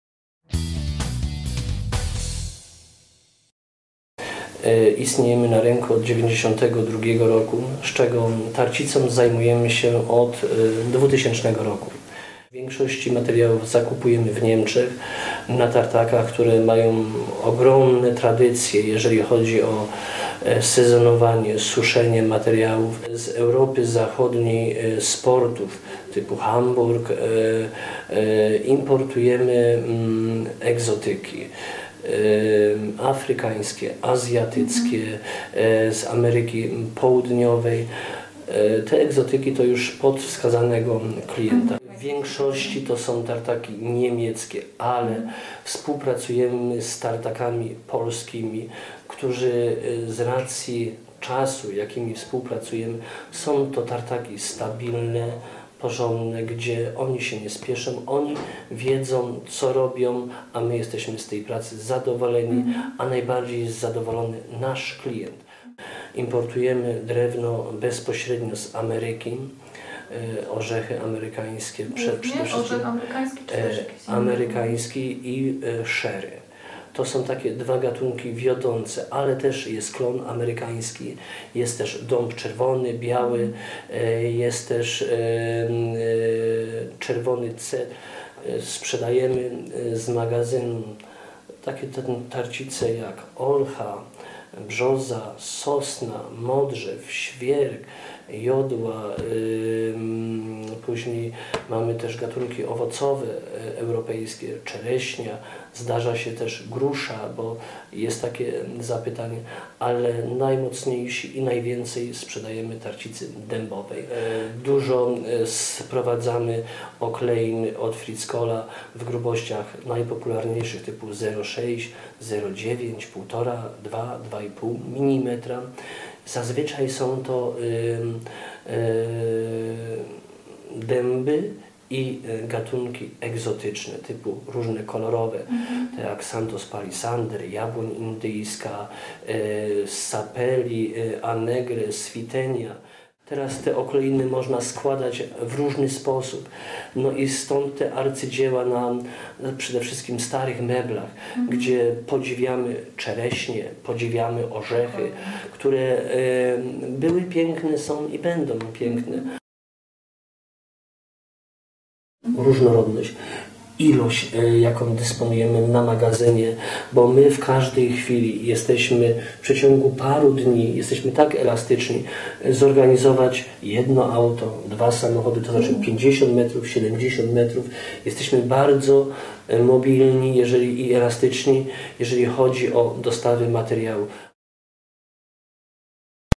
Tarcica wywiad
Wywiad 3 minutowy z dyrektorem firmy Peplinski&Banaszewski.
Styczeń 2013